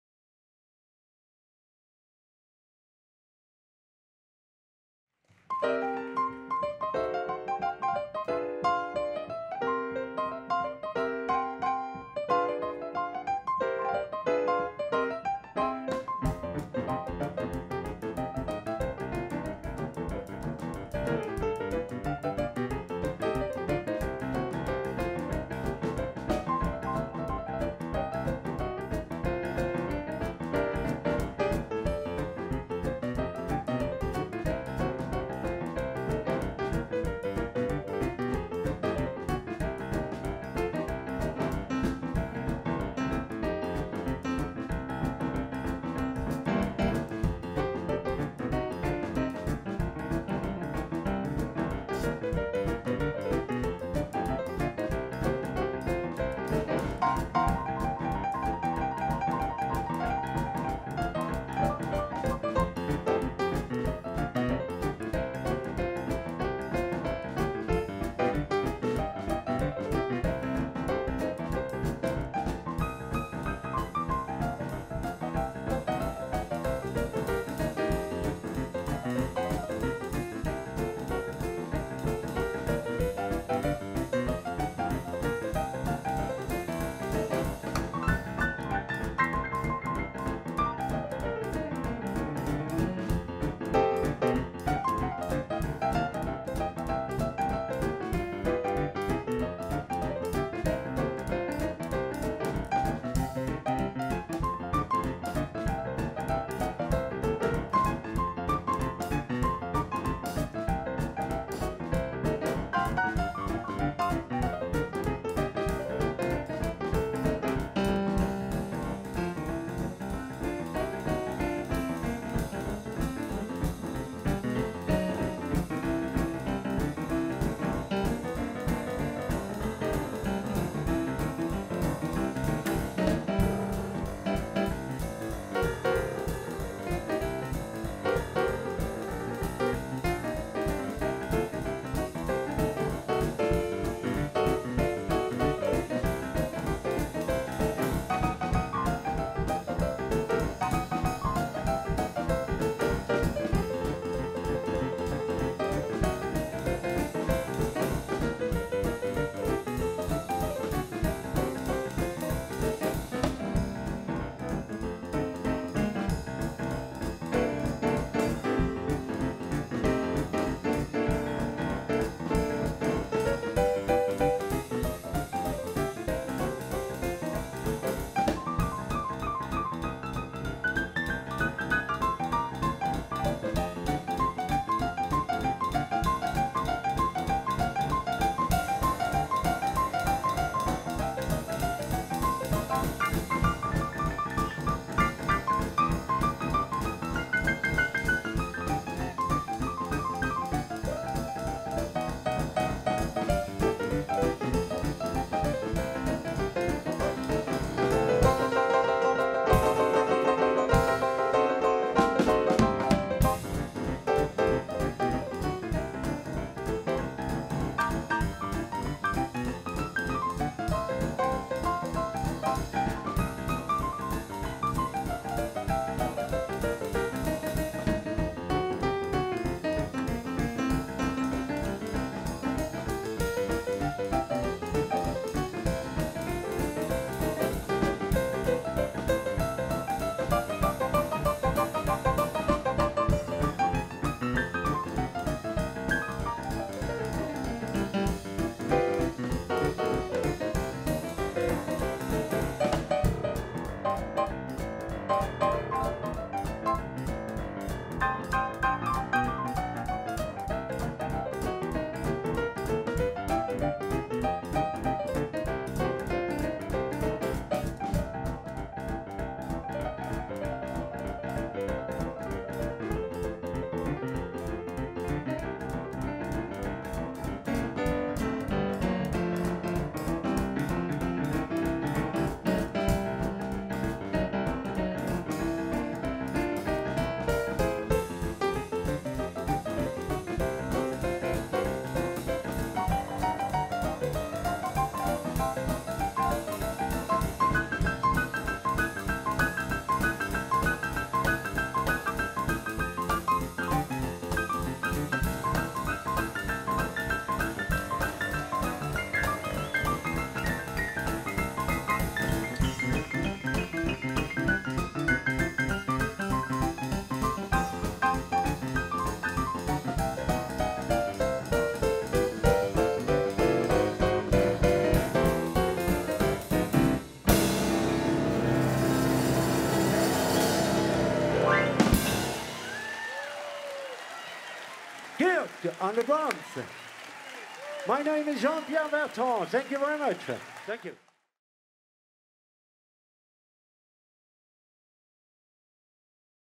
Jean-Pierre Bertrand- 2015 SF Boogie Woogie Festival
Jean-Pierre Bertrand performs at the 2015 San Francisco International Boogie Woogie Festival Donations are welcome